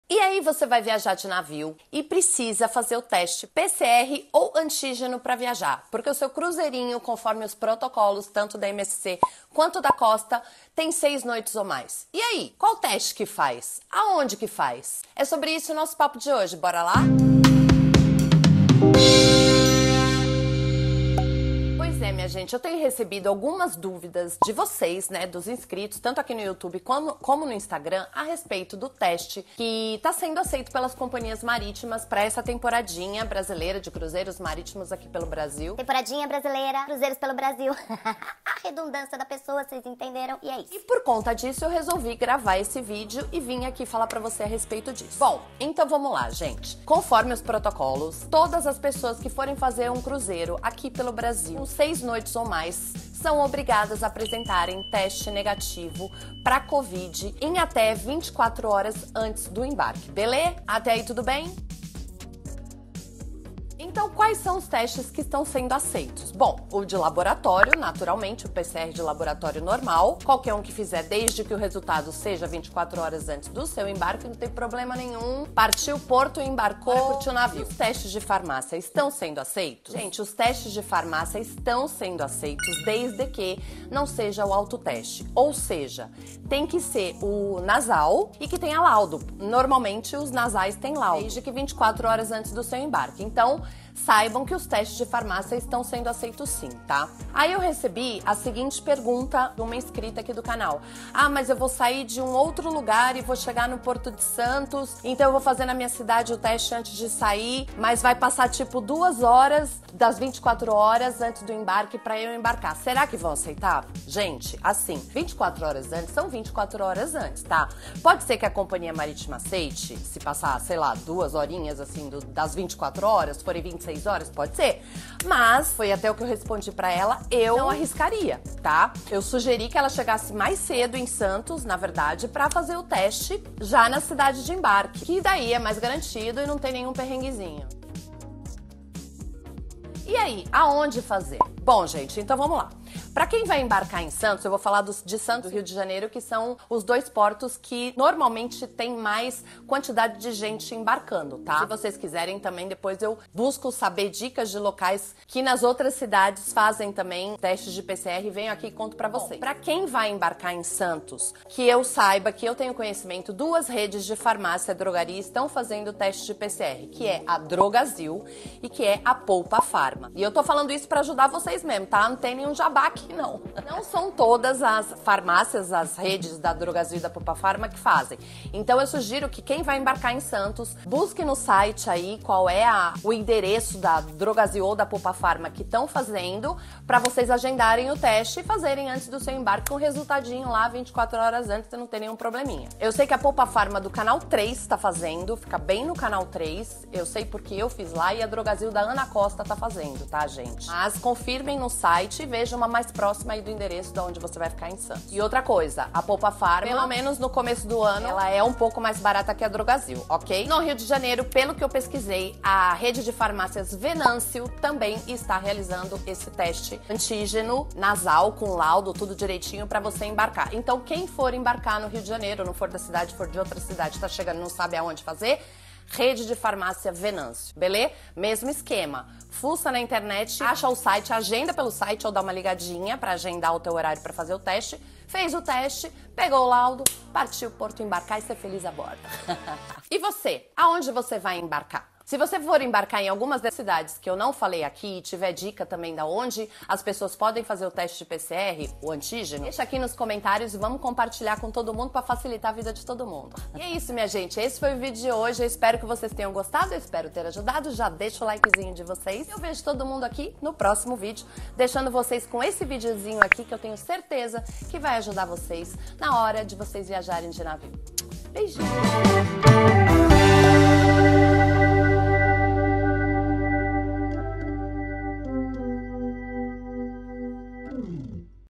TESTE PCR PARA CRUZEIROS - qual fazer? Onde fazer? MSC Seashore MSC Seaview Preziosa Costa Cruzeiros
E aí você vai viajar de navio, em cruzeiros de 6 noites ou mais e, por causa dos protocolos de segurança, precisa apresentar teste PCR ou antígeno com resultado negativo para Covid em até 24 horas antes do embarque.

Mas e aí?

Qual teste PCR / antígeno para cruzeiros marítimos fazer? Qual está sendo aceito pelas Cias Marítimas?

Será que só serve o teste PCR de laboratório? Será que serve o teste de farmácia?

Onde fazer o teste PCR ou antígeno para cruzeiros marítimos em Santos?  E no Rio de Janeiro?

Bora dar um play no vídeo para ficar por dentro de tudo o que tá rolando com os testes PCR ou antígeno para o seu embarque no cruzeiro marítimo, com dicas, inclusive, de onde fazer os testes em Santos e no Rio de Janeiro.

Quer fazer um cruzeiro marítimo?

Você sabia que eu tenho um Guia prático do Cruzeiro Marítimo? Clique para conhecer

Capítulos
00:00 - introdução
00:46 - teste é obrigatório para cruzeiros com 6 noites ou mais
01:01 - quais teste são aceitos pelas Cias Marítimas?
02:28 - onde fazer o teste em Santos e no Rio de Janeiro?
04:00 - finalizando